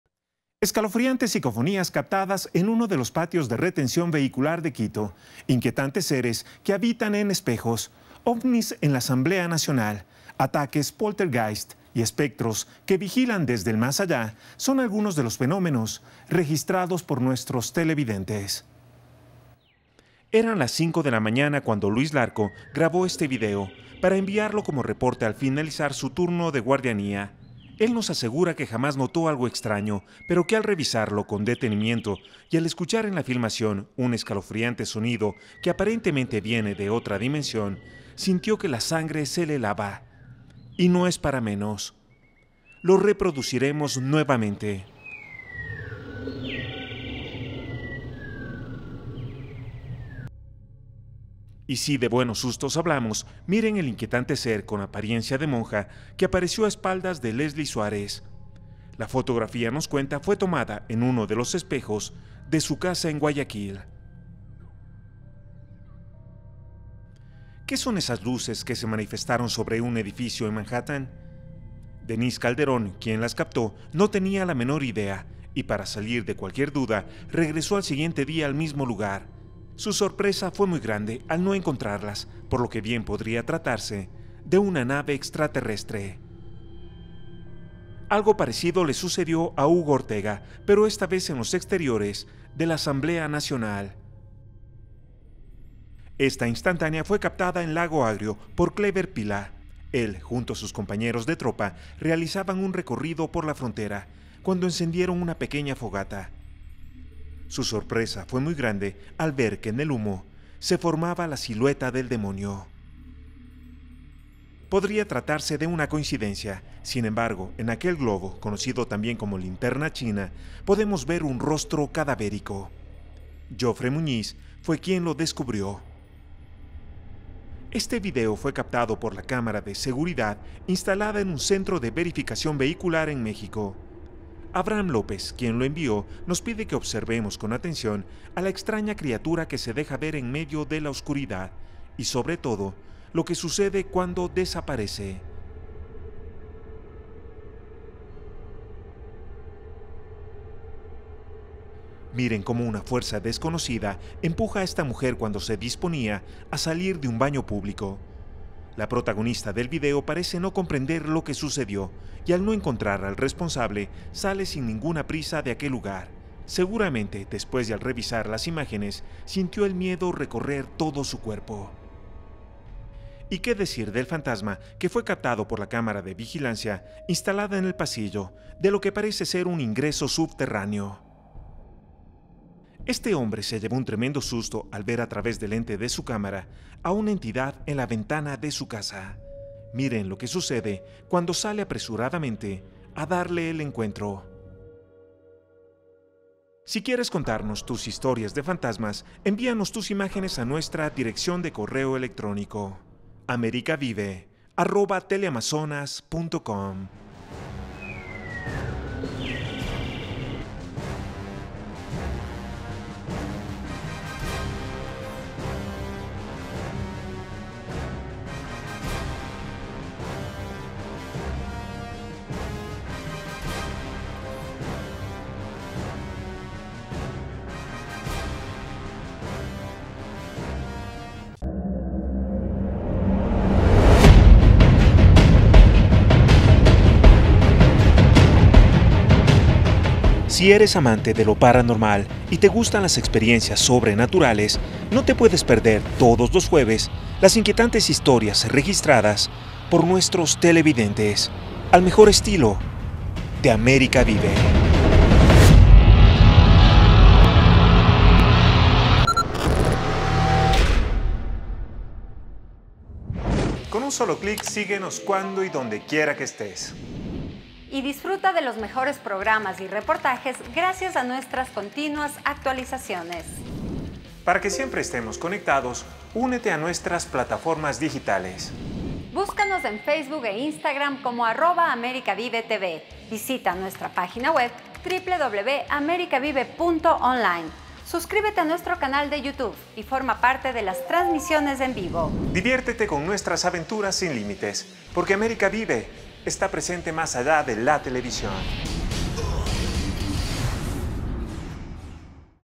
Ovnis en la Asamblea Nacional
Escalofriantes psicofonías captadas en un patio vehicular, inquietantes seres que habitan en espejos, ataques postergáis, espectros que vigilan desde el más allá son algunos de los fenómenos enviados por nuestros seguidores.